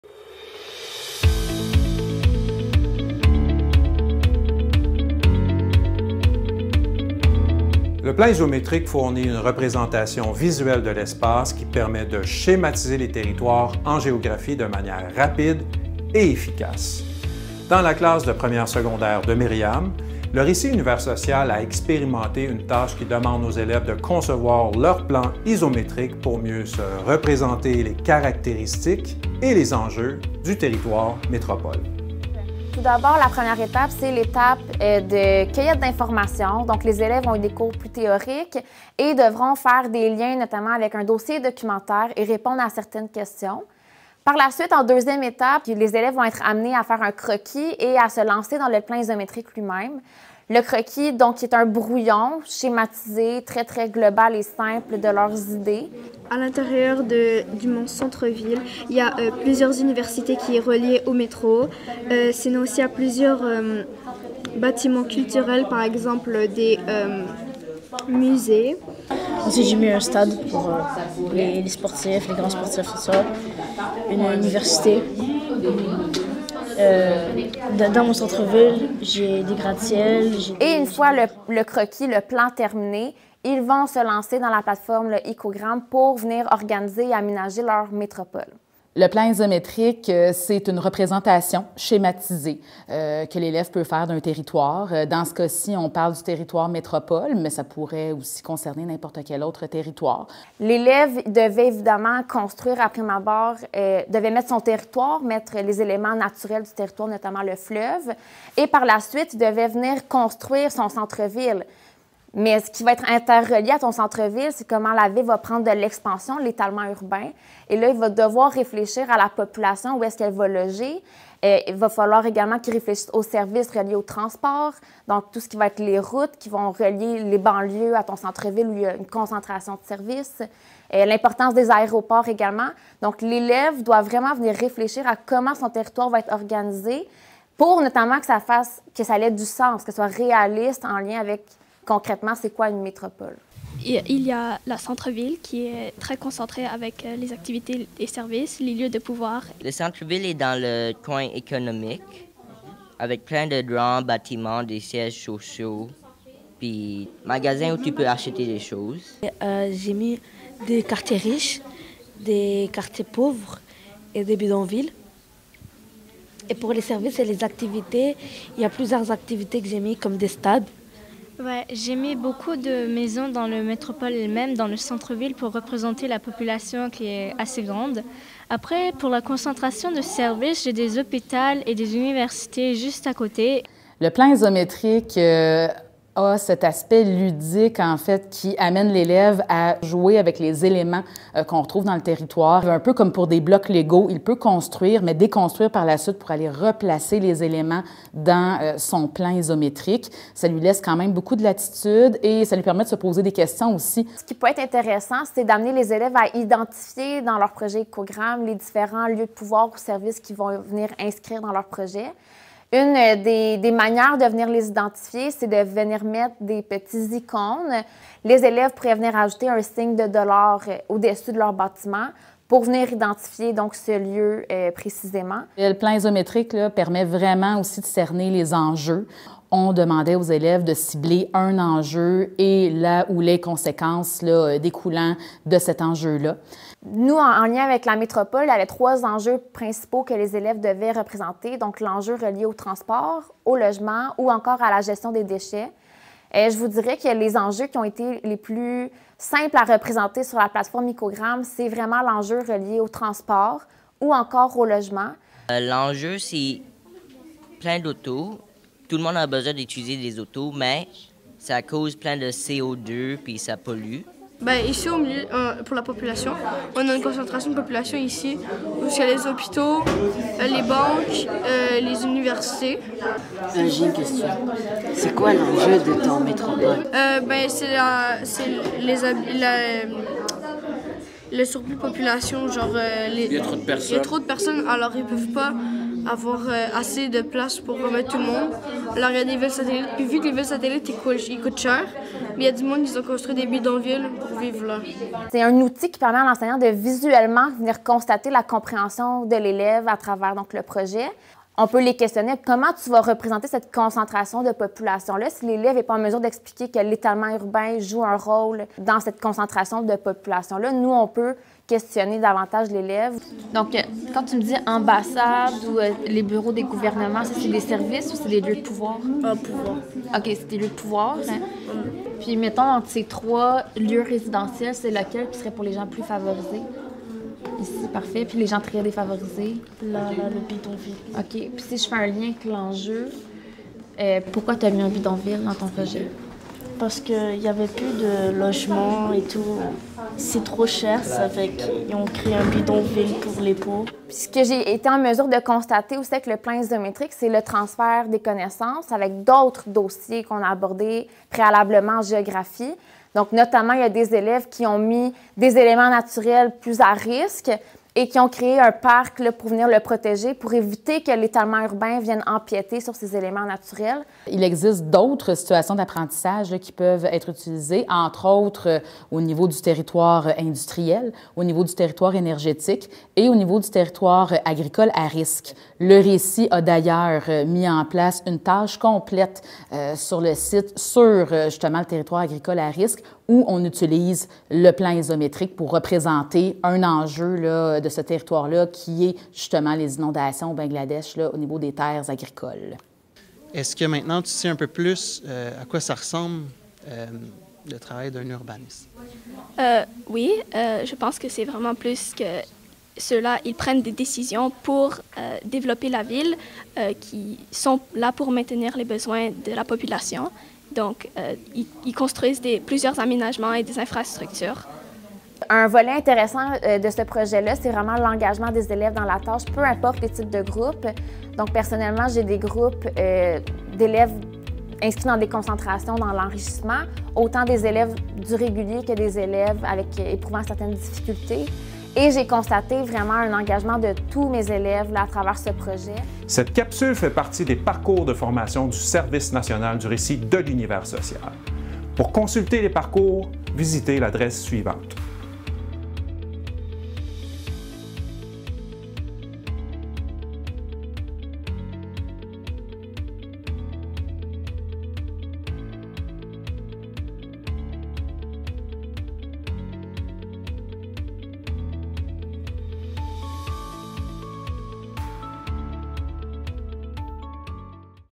Plan isométrique en univers social
Le plan isométrique fournit une représentation visuelle de l’espace qui permet de schématiser les territoires en géographie de manière rapide et efficace. Dans la classe de 1ère secondaire de Myriam, le RÉCIT univers social a expérimenté une tâche qui demande aux élèves de concevoir leur propre plan isométrique pour mieux se représenter les caractéristiques et les enjeux du territoire métropole.